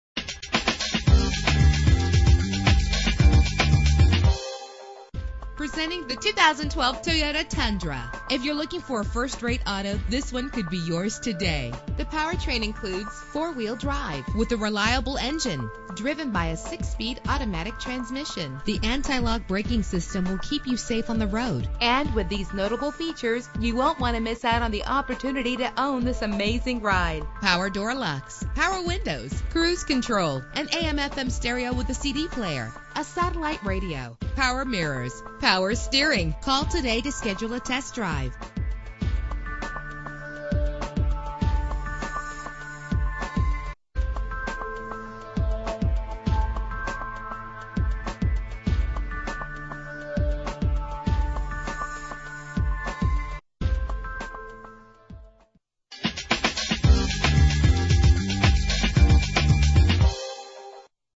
2012 Toyota Tundra - MESA AZ
Year: 2012
Make: Toyota
Model: Tundra
Trim: 4.6L V8
Engine: 4.6 liter V-8
Transmission: 6-Speed Automatic
Color: Barcelona Red
Mileage: 65
Address:
6136 E AUTO LOOP AVE